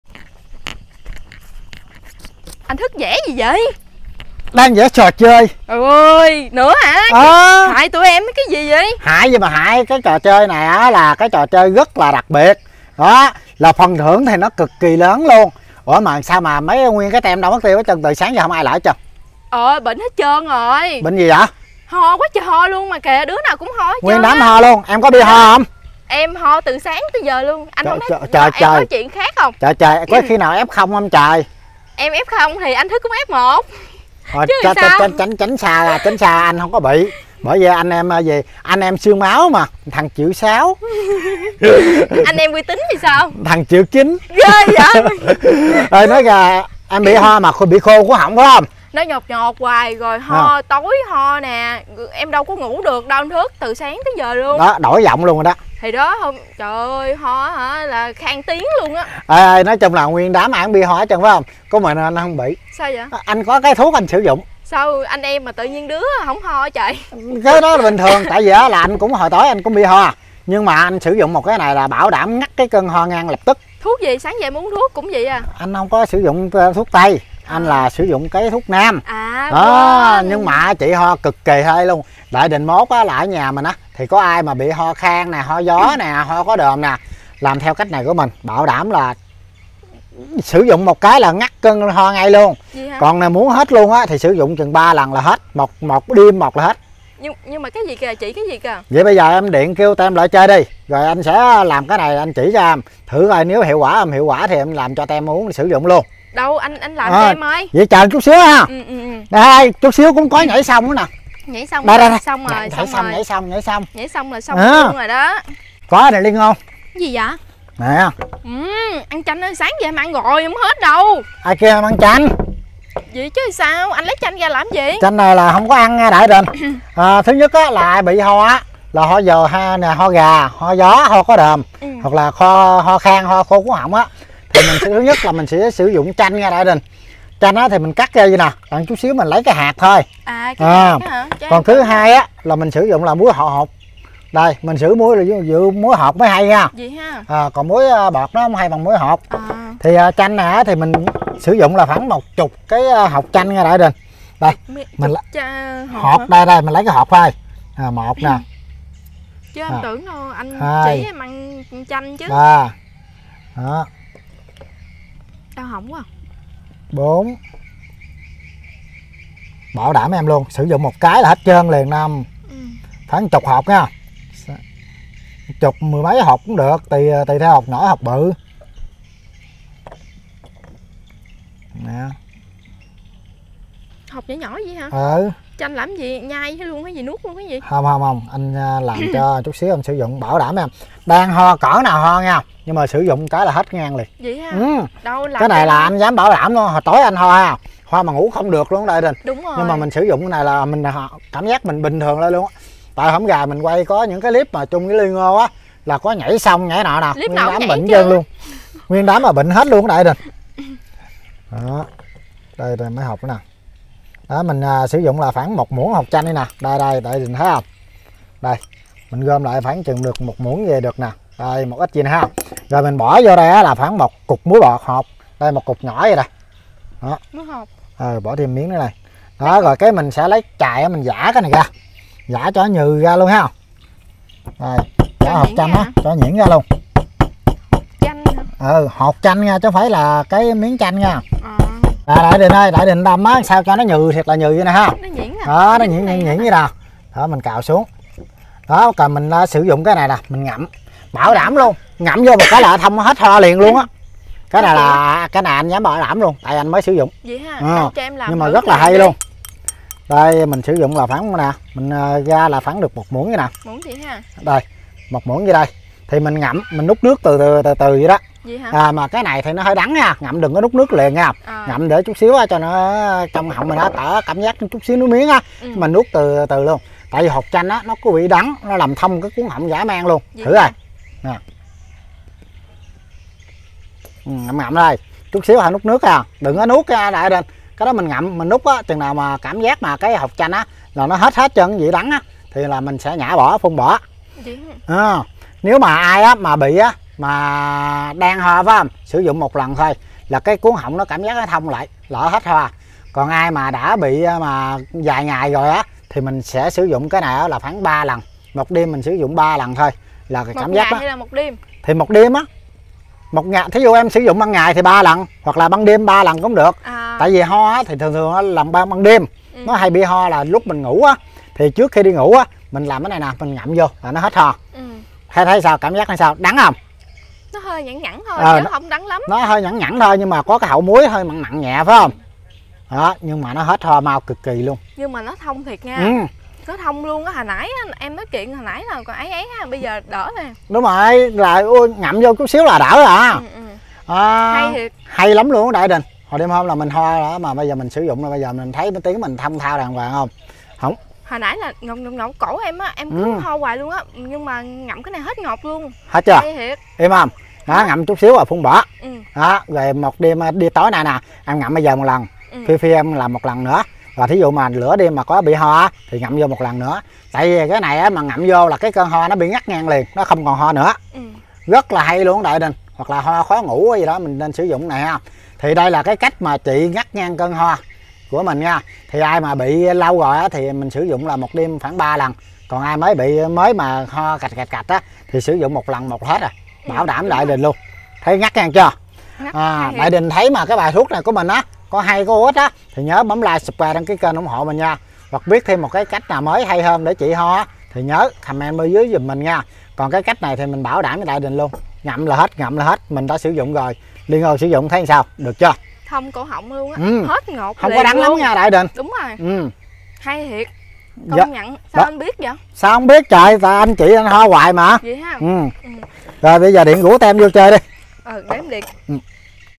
Ngắt Cơn Ho Ngay Lập Tức Chỉ Bằng 1 Biện Pháp Đơn Giản Ai Cũng Làm Được / Công Thức TV
-chúc cả nhà buổi tối vui vẽ hạnh phúc.
© Bản quyền thuộc về Công Thức TV
© Copyright by Công Thức TV ? Do not Reup